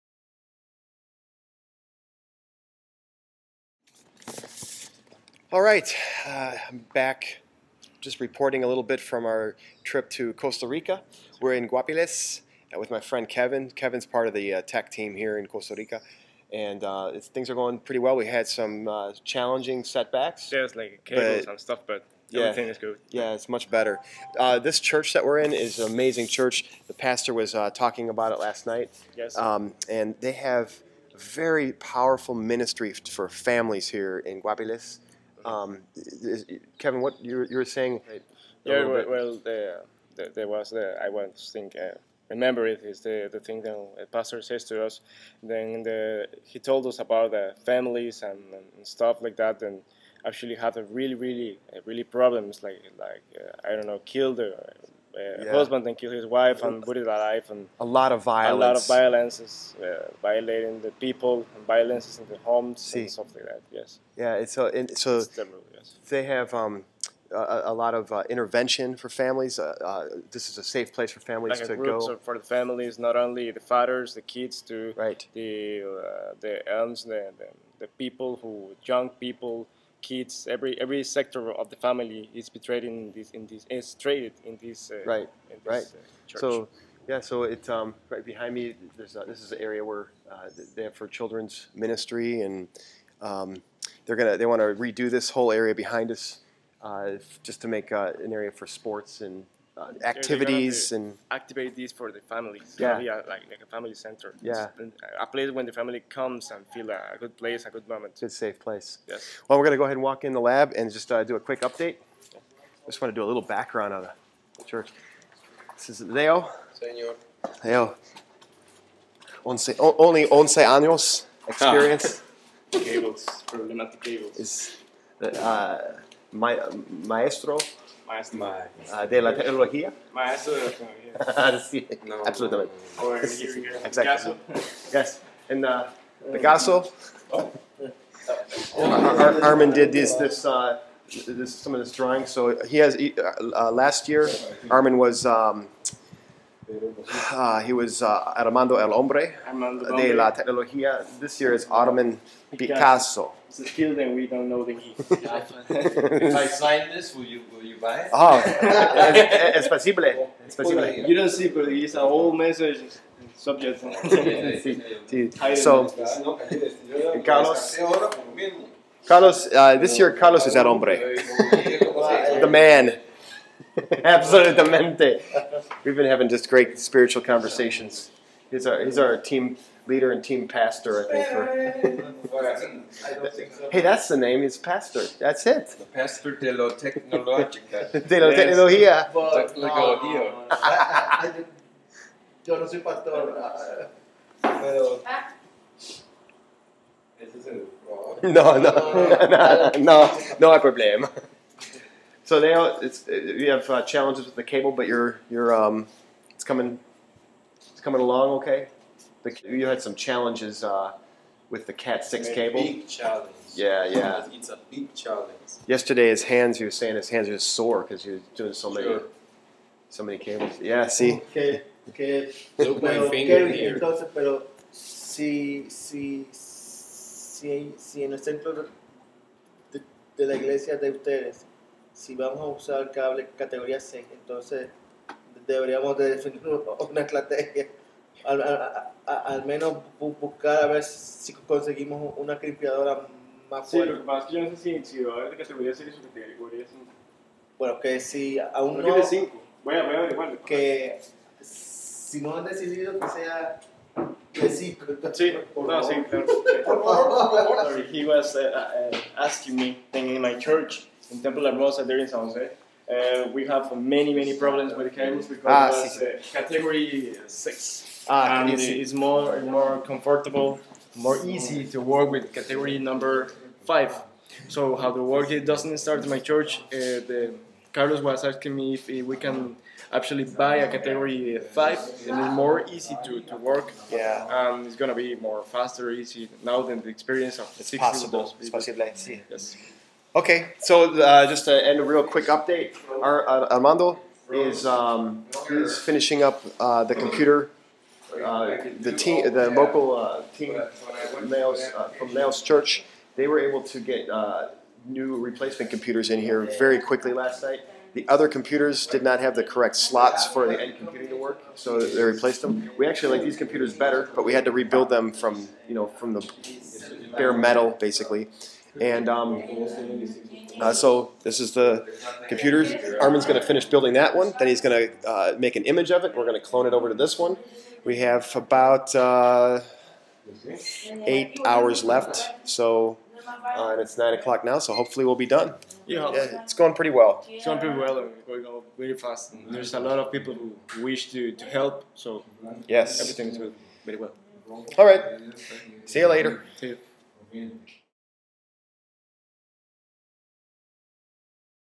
Costa Rica Day 2
On our second day in Costa Rica we are back on track after major setbacks.